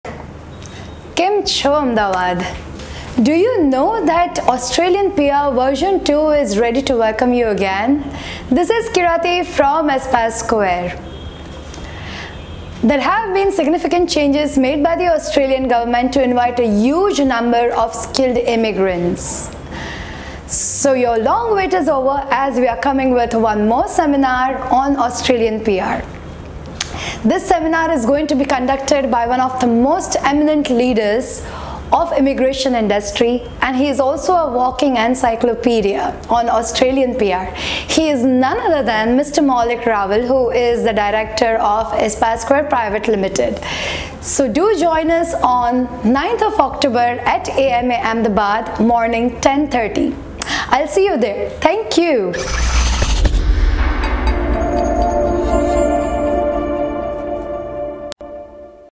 Australia Is Back | Free Australia PR Seminar| @Ahmedabad Management Association| Book Your Seat Now
Planning to Live in Australia? 🇦🇺

Australia has recently declared some amazing announcements that bring smiles to many faces 🥰

✅ The Skilled Migrants Quota increased from 160000 to 195000

Aspire Square brings you a fruitful seminar on Australia PR 🇦🇺 which will give you a road map to move ahead strongly toward your Australian Permanent Residency 😎

✅ About Australia PR Seminar
To resolve all the dilemmas Aspire Square brings an informative session from the experts of the industry 🤗 Mr. Maulik Raval

✅The premium purpose of the seminar is;
👉 To spread awareness about recent changes in Australian immigration.
👉 How you may qualify for the Australia PR?
👉 What subclass you should opt?
👉 Clarification and classification of occupation.
👉 Description of your eligibility to your selection scope.
👉 Solution of your all queries, questions, and confusions.

✅ Grab your free seat today